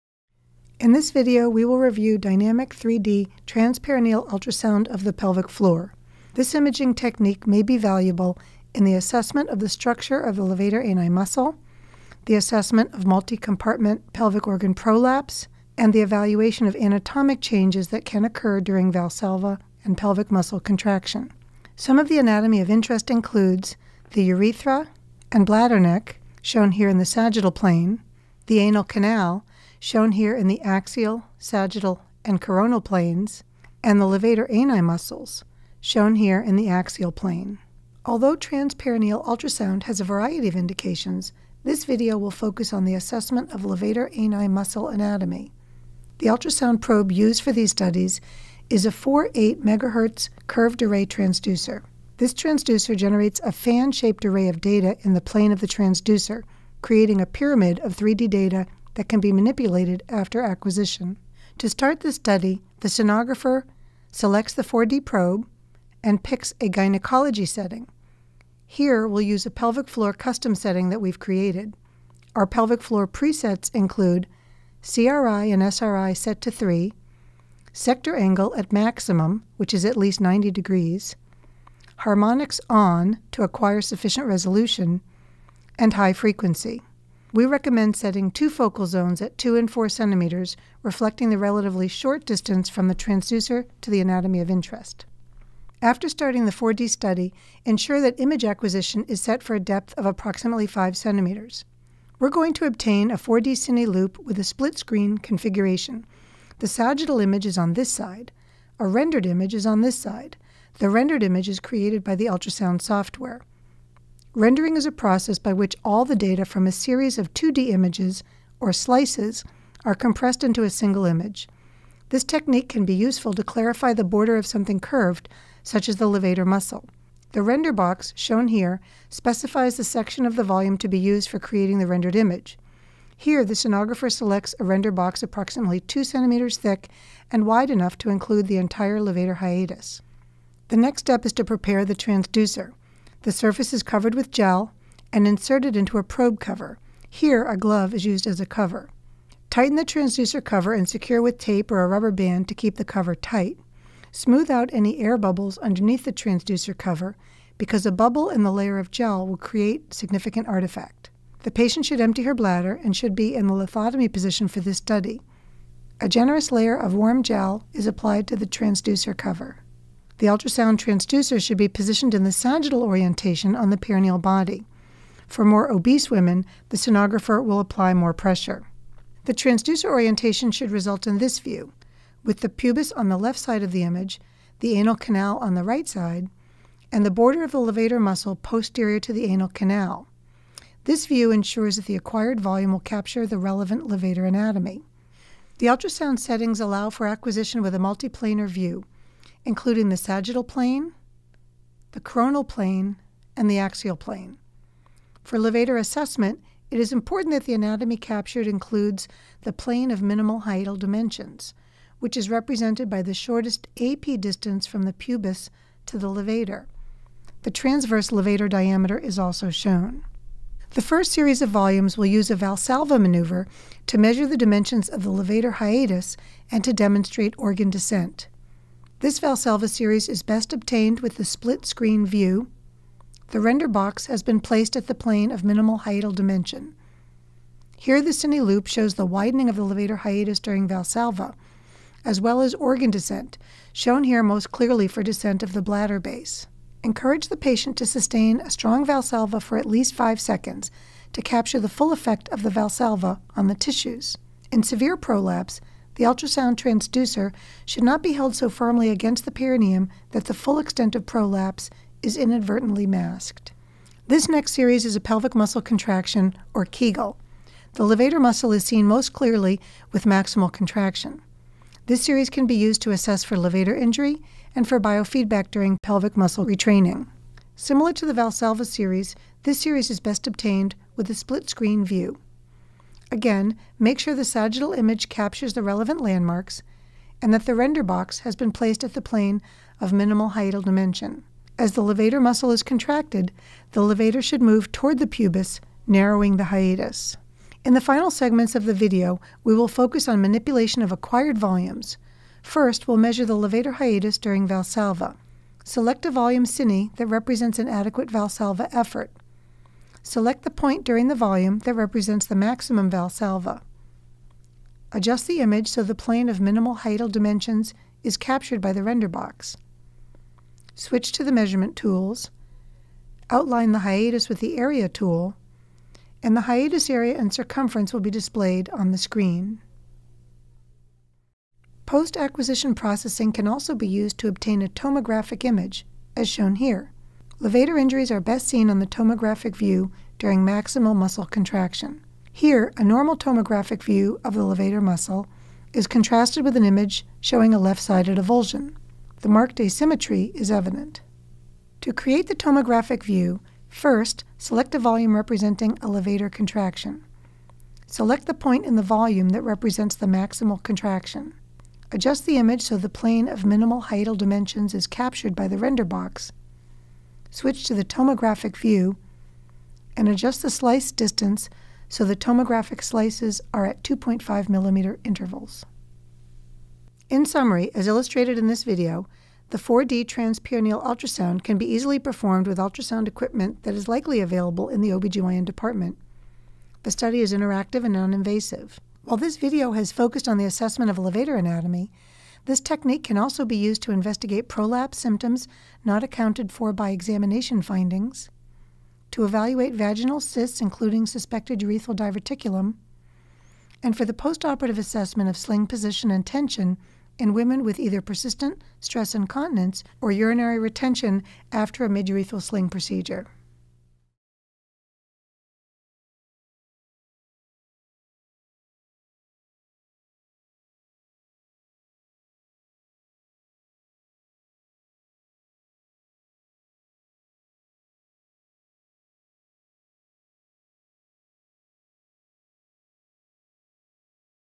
4D Transperineal Ultrasound of the Female Pelvic Floor
This video reviews the technique for dynamic 3-D transperineal ultrasound of the pelvic floor. This imaging technique may be valuable in the assessment of the levator ani muscle in women with pelvic floor disorders and in the evaluation of pelvic organ prolapse.